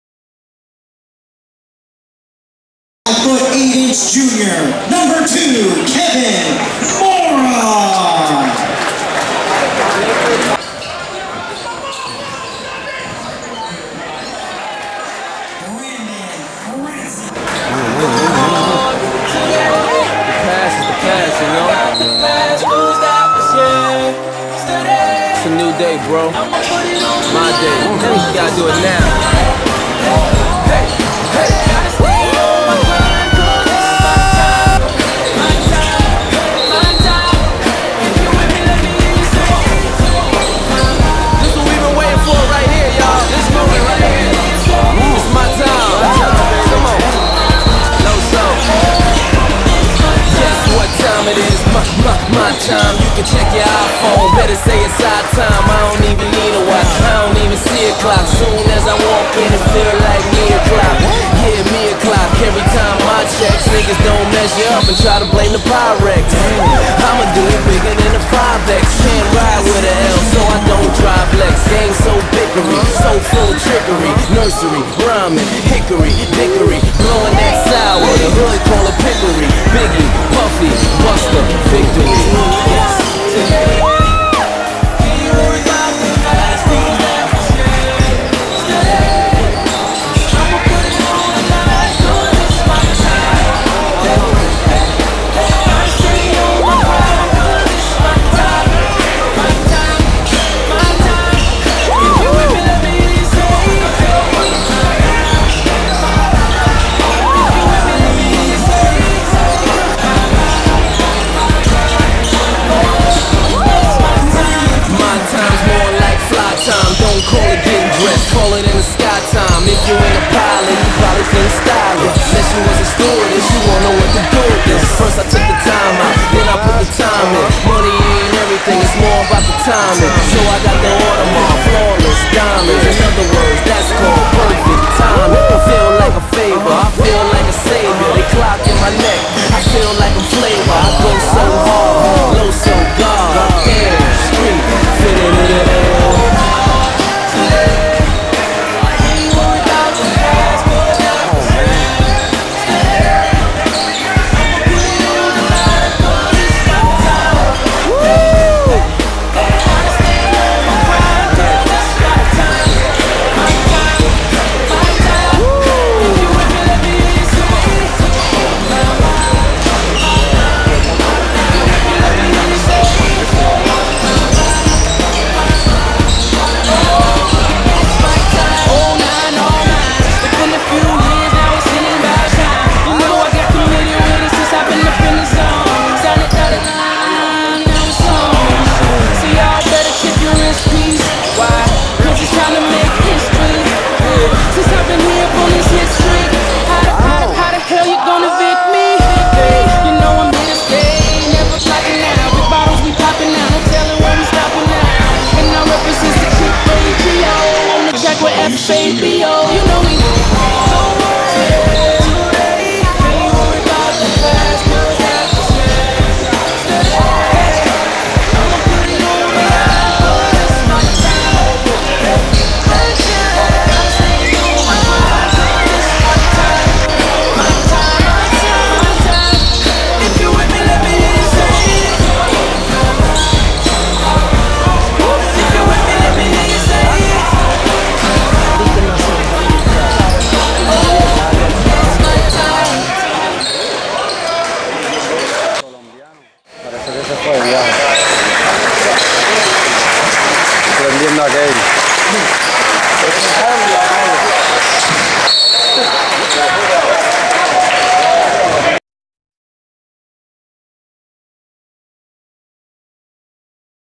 Natural Point Guard KJ Maura
Highlights City Of Palms Tournament Point Guard KJ Maura of ACD